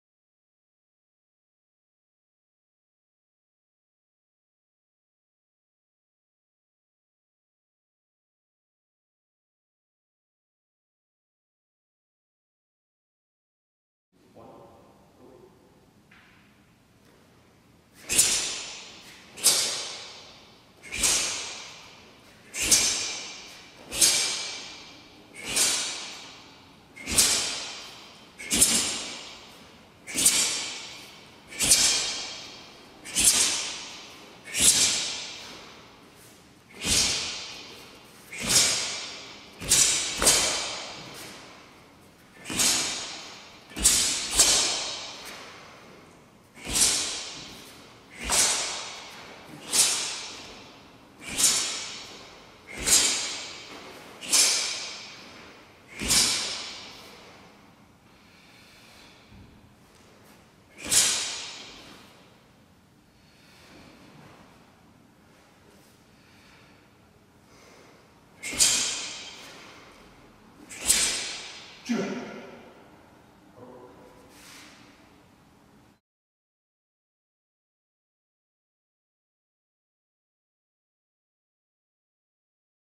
Joong-Gun - Pattern
Joong-Gun, ITF TaeKwon-Do Pattern performed for instructional purposes by Mr. Joël Denis - Head Instructor V Degree Black Belt